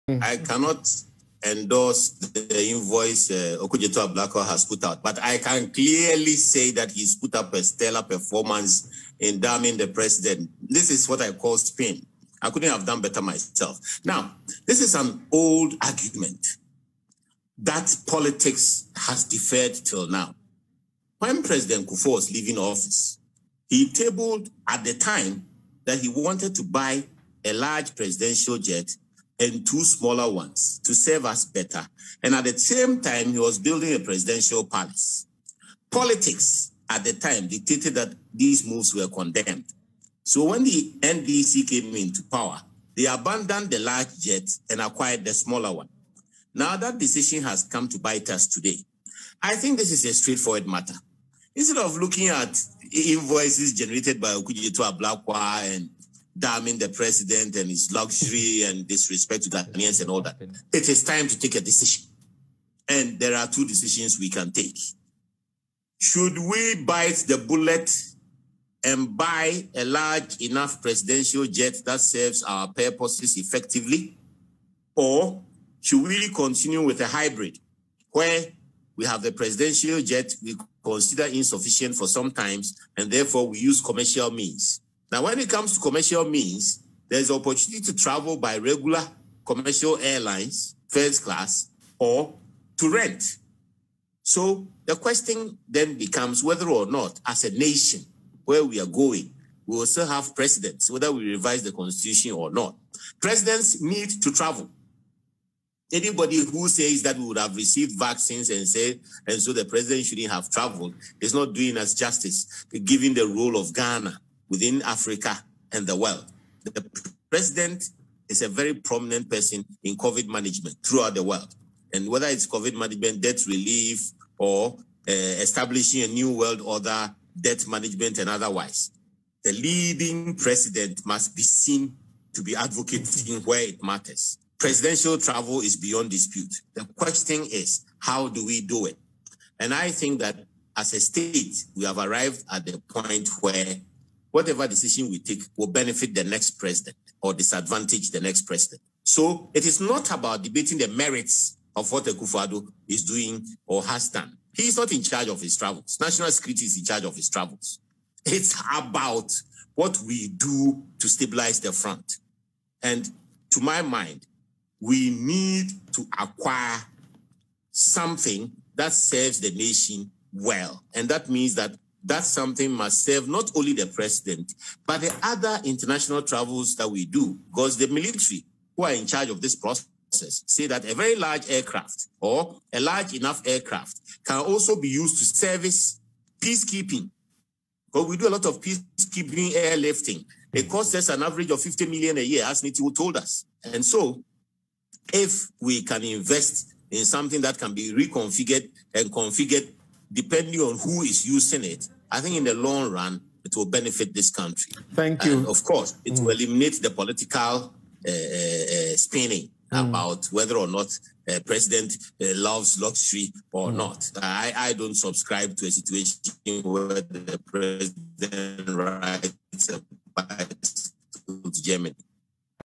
Government should buy a larger jet - Yaw Buaben Asamoah
The Communications Director of the New Patriotic Party, Yaw Buaben Asamoah has suggested that government should go ahead to acquire a larger Presidential jet.